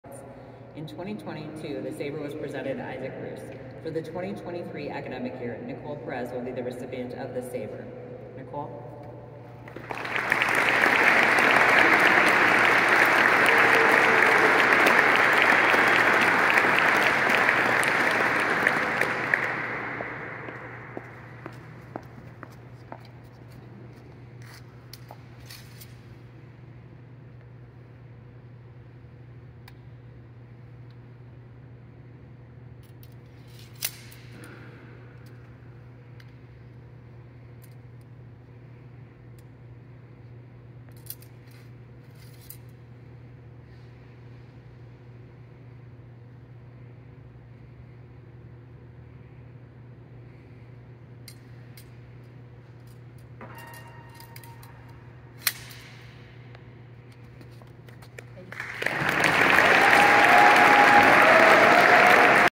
Norwich University ECE Sword Ceremony
Passing the honor of carrying the Barry E. Wright Memorial Saber from graduating senior Isaac Ruse to rising senior Nicole Perez. at the 2023 Norwich Engineering Convocation